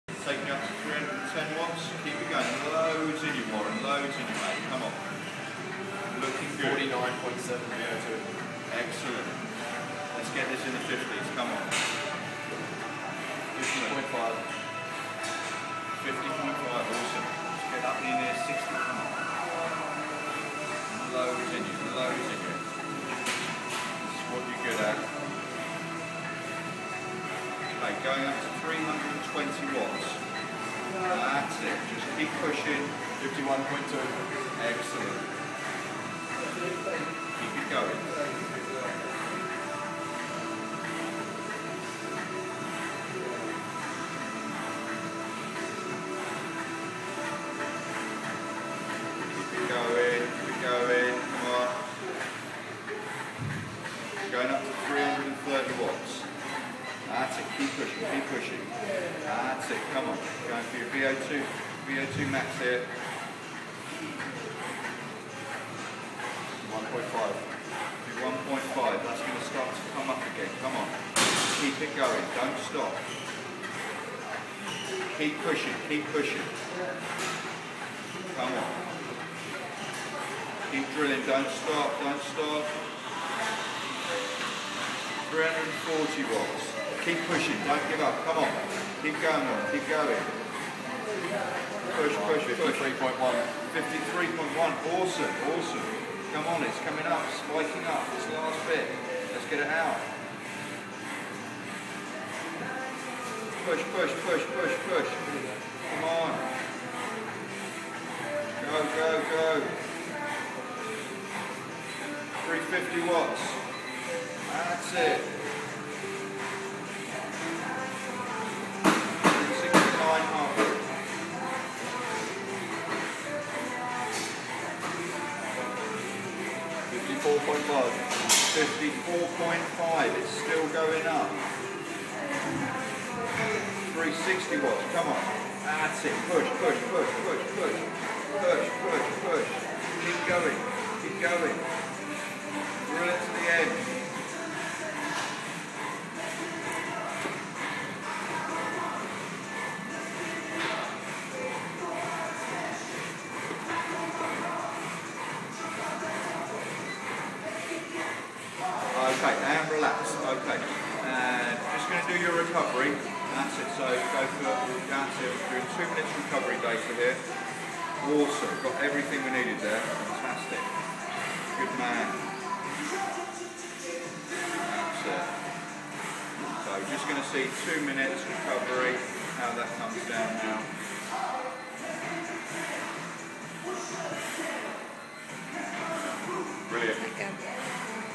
VO2 Max Metabolic Testing for Warren Pole (Cycling Journalist)
Pro-VO2 expert fitness testers Pat Leahy & Adrian Mastalerski putting freelance cycling & triathlon journalist Warren Pole through his VO2Max metabolic fitness test to set his training zones for his Etape cycling training 2011.

The test was performed at the Pro-VO2 multisport lab at Fit Rooms, Fulham with Maxifuel nutrition expert Lynn Clay assessing Warrens nutrition plan from the results.

More about VO2Max Testing, Training Plans and Fitness Coaching for sport can be found out at the website ...

... London's No1 Cycling & Triathlon coaching company!